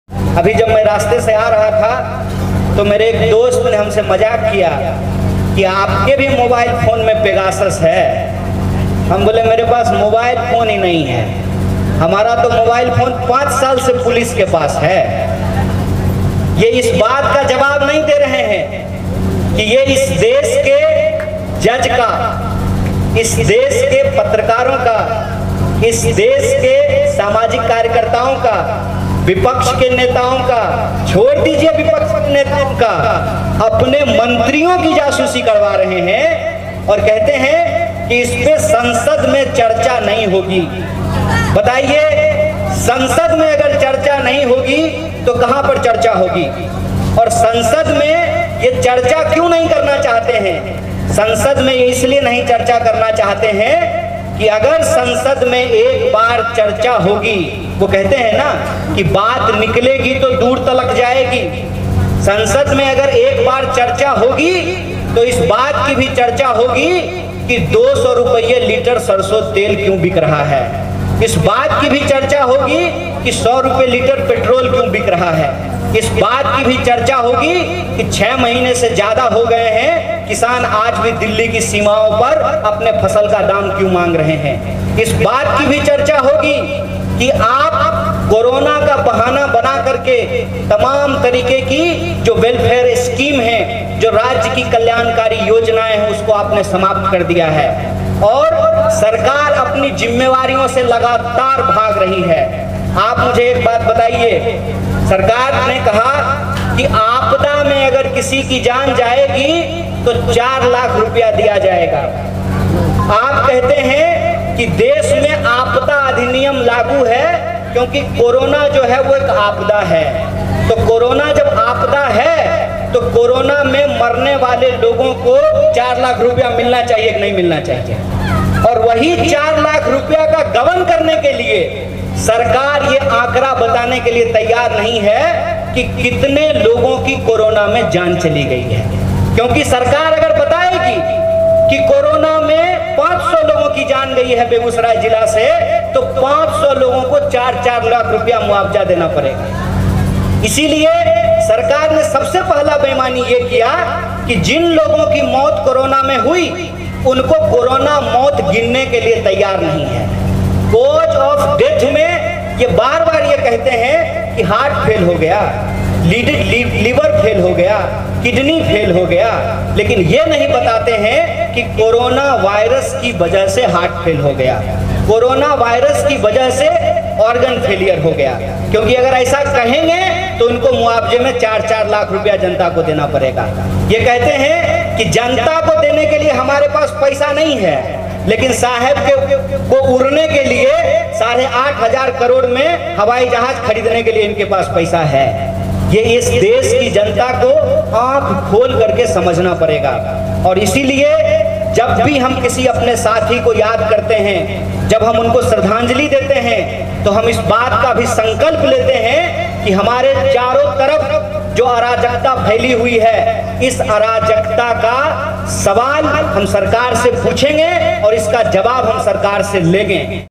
Kanhaiya Kumar का प्रधानमंत्री MODI पर तंज, पूछा संसद में pegasus पर चर्चा क्यों नहीं।
Dr Kanhaiya Kumar Speech.